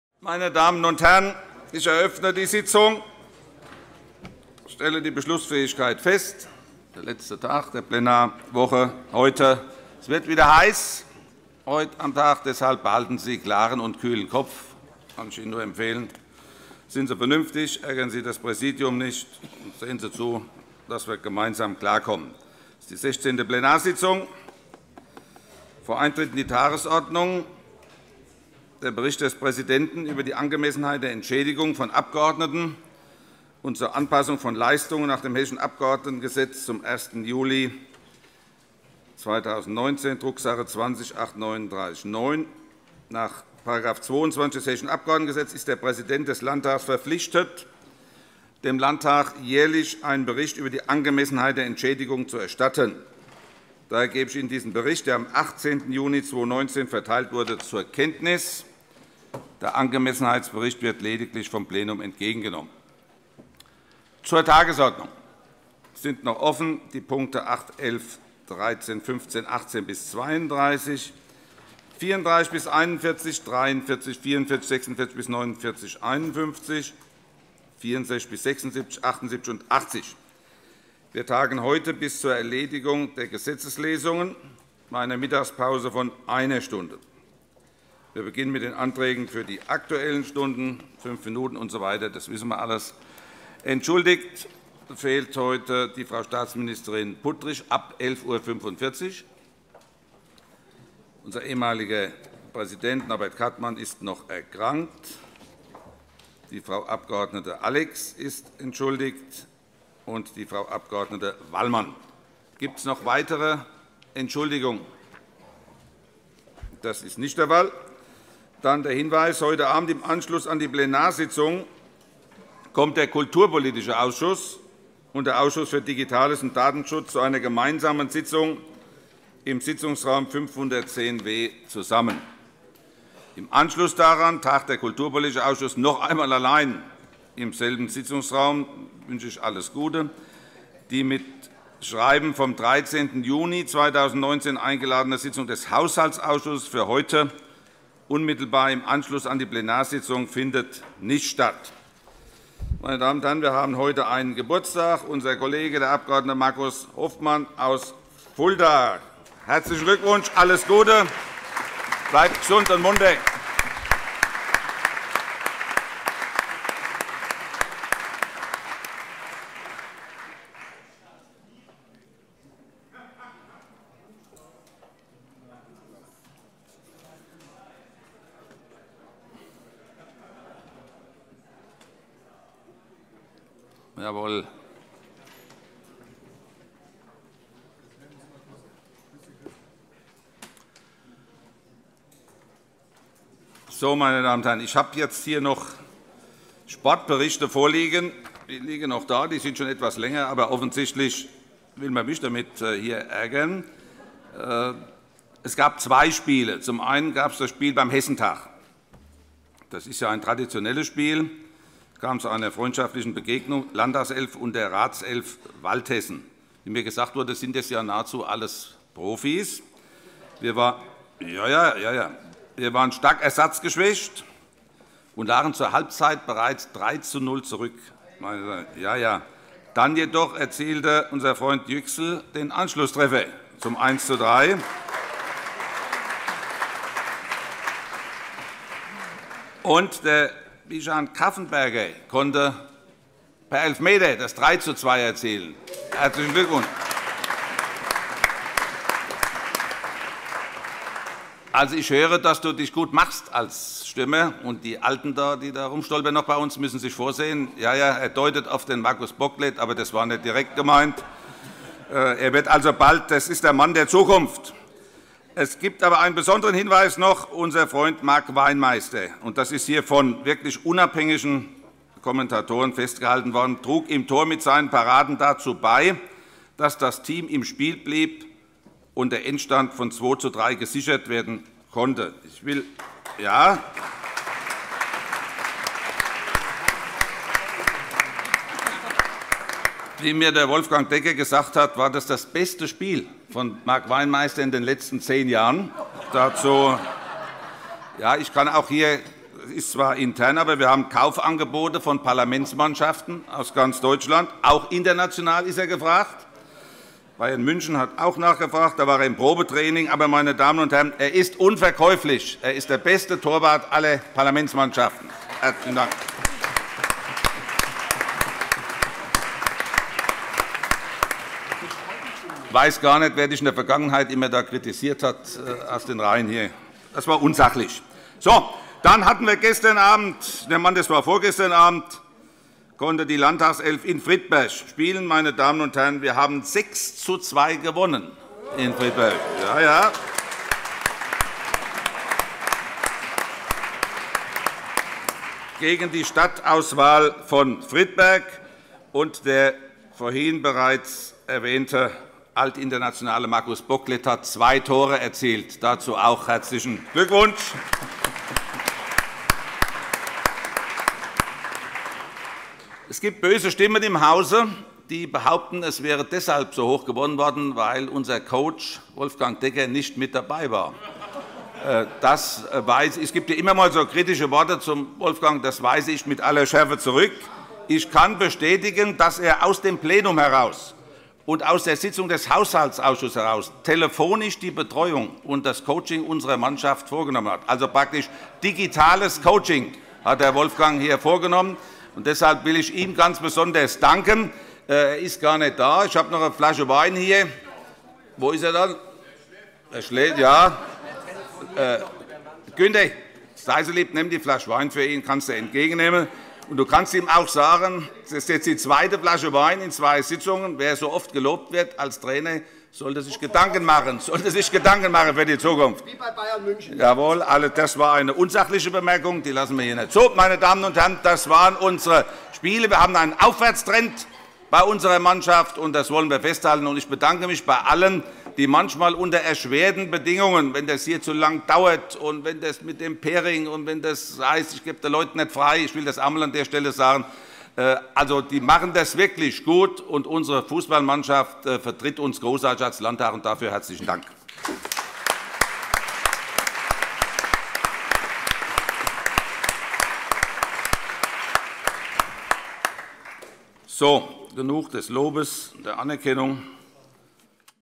Amtliche Mitteilungen, Bericht Landtagspräsident - 19.06.2019 - 16. Plenarsitzung
Bericht des Präsidenten des Landtags nach § 22 des Hessischen Abgeordnetengesetzes über die Angemessenheit der Entschädigungen von Abgeordneten und zur Anpassung
von Leistungen zum 1. Juli 2019 – Drucks. 20/839 neu